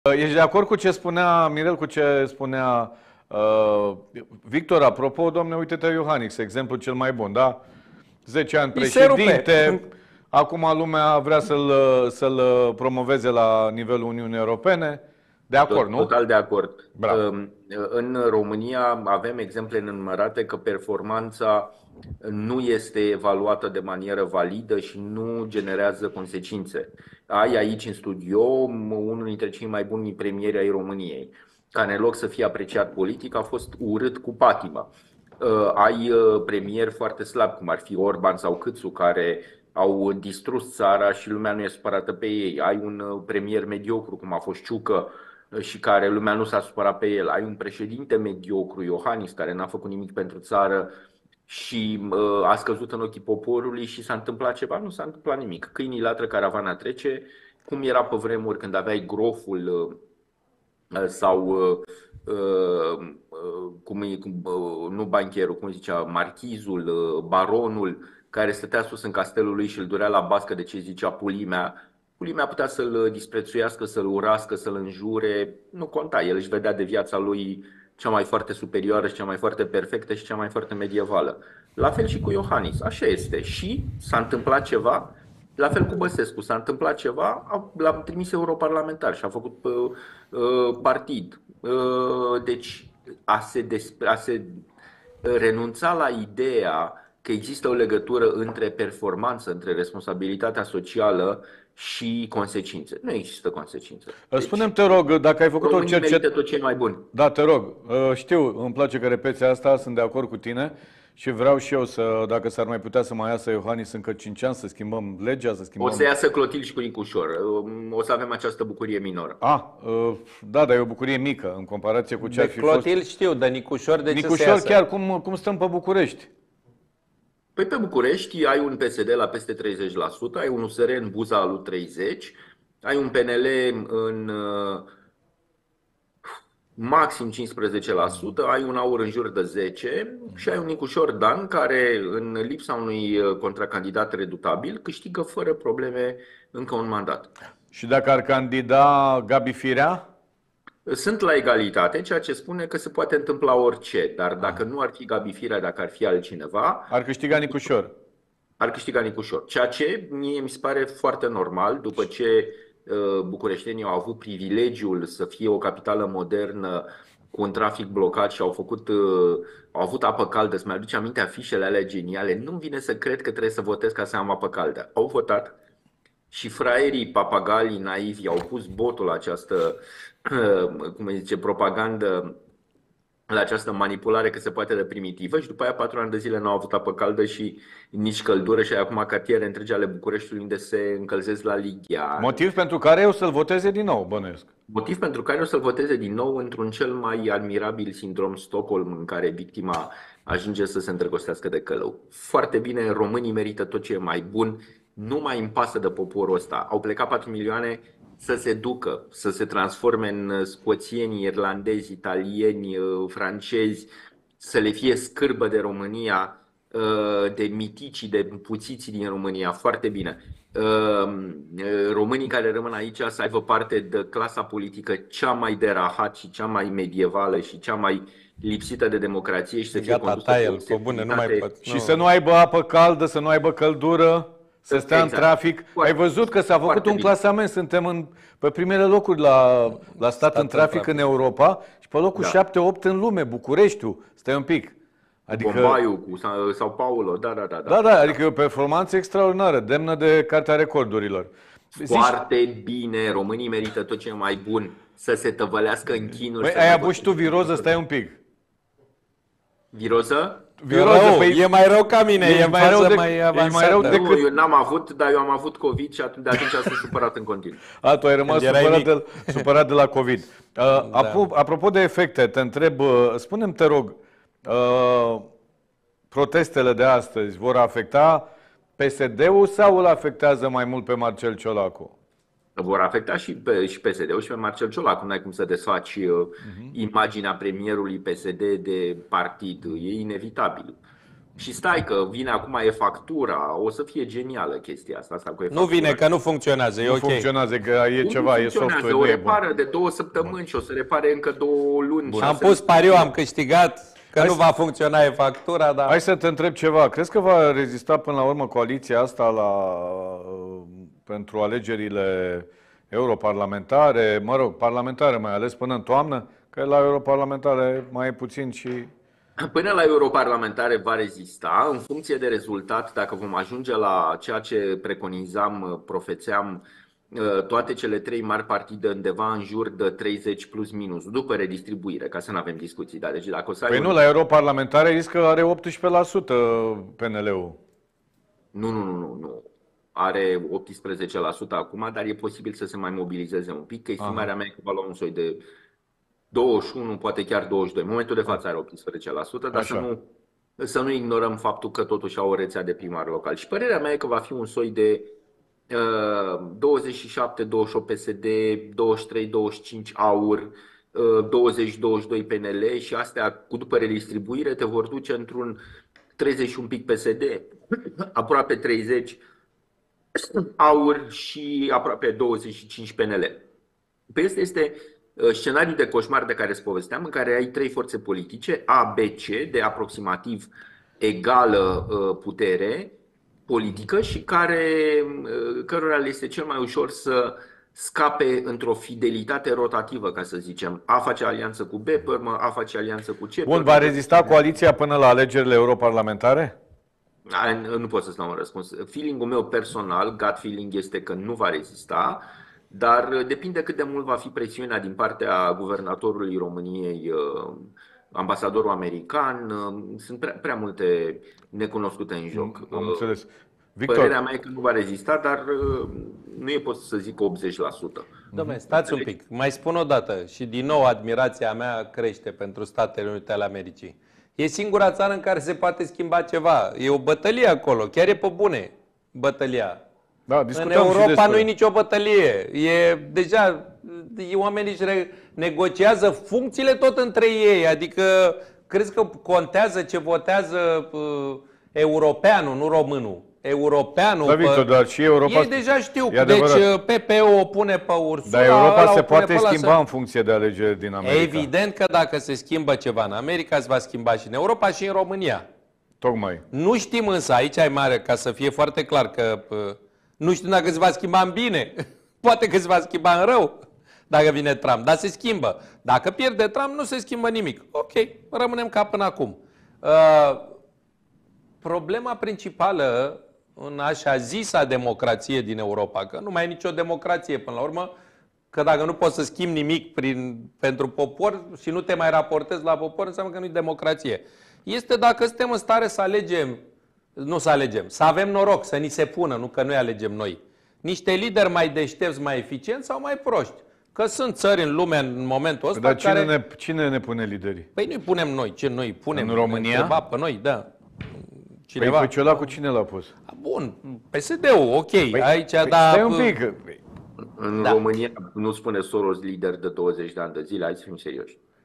Mirel Palada: "Protestele din zilele acestea vor AFECTA PSD"
Invitații zilei au fost Victor Ponta, fost premier al României și Mirel Palada, sociolog. Cei trei au abordat diverse subiecte, printre care protestele din ultimele zile, alegerile din America, dar și alegerile din 2024 din România.